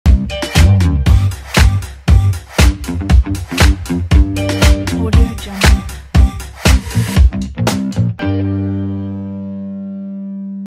Rhythmic Stomp Opener
This project will help you to create dynamic, trendy slideshow, even if you’re new in After Effects.

Project features:

    Color control

    Fast render

    100 % After Effects